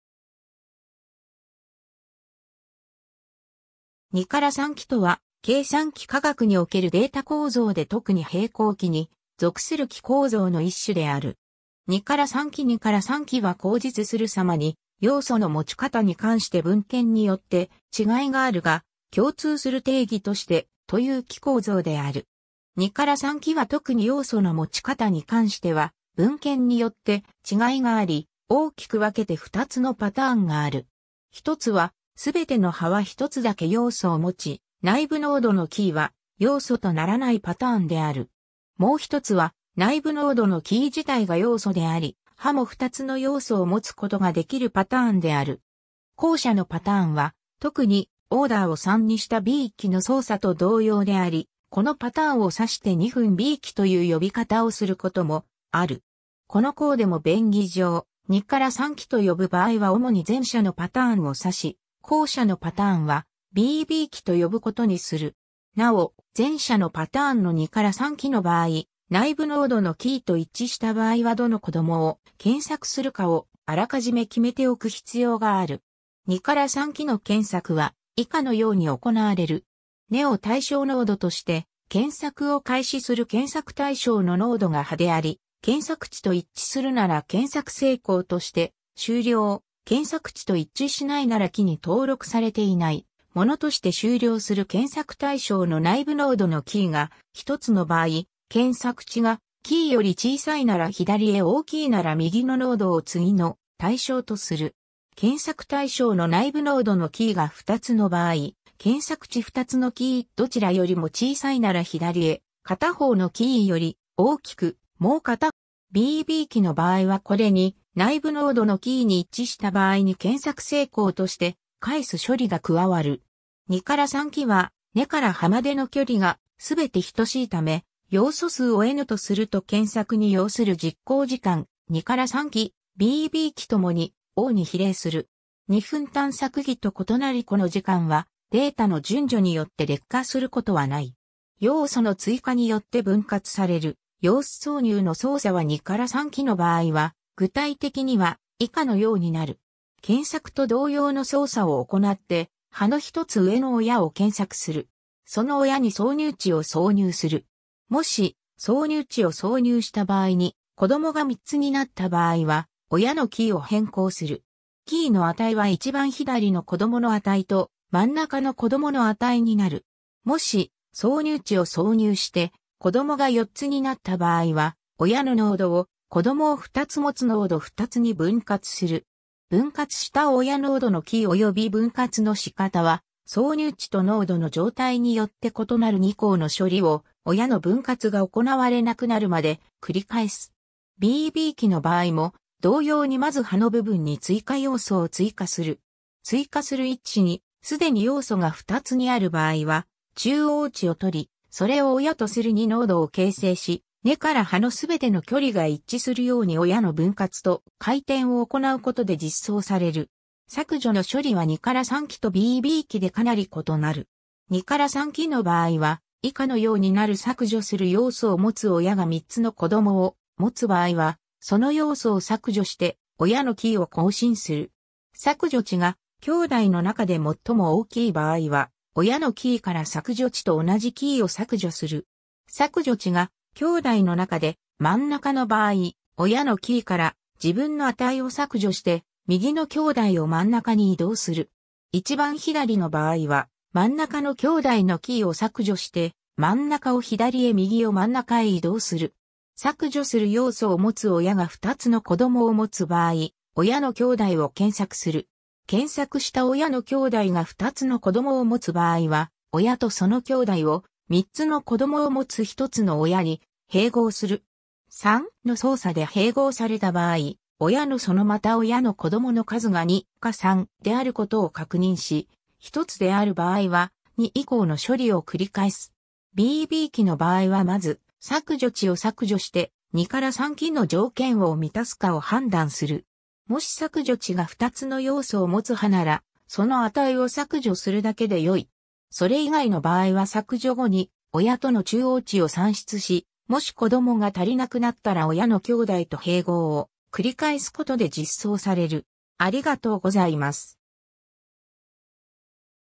2-3木
2-3木（2-3き、英: 2-3 tree）とは計算機科学におけるデータ構造で特に平衡木（balanced tree）に属する木構造の一種である。
 2-3木（全ての要素を葉に持つパターン） 2-3木は後述する様に要素の持ち方に関して文献によって違いがあるが、共通する定義として、 という木構造である。
2-3木は特に要素の持ち方に関しては文献によって違いがあり、大きく分けて二つのパターンがある。
一つは全ての葉は一つだけ要素を持ち、内部ノードのキーは要素とならない（すなわち全ての要素は葉に格納される）パターンである。
もう一つは内部ノードのキー自体が要素であり、葉も2つの要素を持つことができるパターンである。
後者のパターンは特にオーダーを 3にしたB木の操作と同様であり、このパターンを指して2分B木（BB木; Binary B-tree）という呼び方をすることもある。
この項でも便宜上、2-3木と呼ぶ場合は主に前者のパターンを指し、後者のパターンはBB木と呼ぶことにする。
なお前者のパターンの2-3木の場合、内部ノードのキーと一致した場合はどの子供を検索するかを、あらかじめ決めておく必要がある（以降のパターンでは同じ値の場合、より右の子供を検索するとする）。
 2-3木の検索は以下のように行われる。
 根を対象ノードとして検索を開始する 検索対象のノードが葉であり、検索値と一致するなら検索成功として終了、検索値と一致しないなら木に登録されていないものとして終了する 検索対象の内部ノードのキーが一つの場合、検索値がキーより小さいなら左へ大きいなら右のノードを次の対象とする。
 検索対象の内部ノードのキーが二つの場合、検索値2つのキーどちらよりも小さいなら左へ、片方のキーより大きくもう片方のキーより小さいなら真ん中へ、どちらよりも大きい場合は右のノードを次の対象とする 2.以下を繰り返す。
 BB木の場合はこれに、内部ノードのキーに一致した場合に検索成功として返す処理が加わる。
2-3木は根から葉までの距離が全て等しいため、要素数を N とすると検索に要する実行時間 2-3木、BB木共に O (log N) に比例する。
2分探索木と異なりこの時間は、データの順序によって劣化することはない。
 要素の追加によって分割される様子 挿入の操作は2-3木の場合は具体的には以下のようになる。
 検索と同様の操作を行って葉の一つ上の親を検索する。
 その親に挿入値を挿入する。
 もし、挿入値を挿入した場合に、子供が3つになった場合は、親のキーを変更する。
キーの値は一番左の子供の値と、真ん中の子供の値になる。
 もし、挿入値を挿入して子供が 4つになった場合は、親のノードを、子供を二つもつノード二つに分割する。
分割した親ノードのキーおよび分割の仕方は挿入値とノードの状態によって異なる（右図参照） 2以降の処理を親の分割が行われなくなるまで、繰り返す。
 BB木の場合も同様にまず葉の部分に追加要素を追加する。
追加する位置にすでに要素が2つにある場合は中央値をとり、それを親とする2ノードを形成し、根から葉の全ての距離が一致するように親の分割と、回転を行うことで実装される（詳しくはB木を参照）。
 削除の処理は2-3木とBB木でかなり異なる。
2-3木の場合は以下の様になる 削除する要素を持つ親が3つの子供を持つ場合は、その要素を削除して、親のキーを更新する。
削除値が兄弟の中でもっとも大きい場合は、親のキーから削除値と同じキーを削除する。
削除値が兄弟の中で真ん中の場合、親のキーから自分の値を削除して右の兄弟を真ん中に移動する。
一番左の場合は真ん中の兄弟のキーを削除して真ん中を左へ右を真ん中へ移動する。
 削除する要素を持つ親が2つの子供を持つ場合、親の兄弟を検索する。
 検索した親の兄弟が2つの子供を持つ場合は親とその兄弟を、3つの子供を持つ1つの親に併合する。
 3.の操作で併合された場合、親のそのまた親の子供の数が 2.か 3.であることを確認し、1つである場合は、2以降の処理を繰り返す。
 BB木の場合はまず、削除値を削除して、2-3木の条件を満たすかを判断する。
もし削除値が2つの要素をもつ葉なら、その値を削除するだけでよい。
それ以外の場合は削除後に親との中央値を算出し、もし子供が足りなくなったら親の兄弟と併合を繰り返すことで実装される。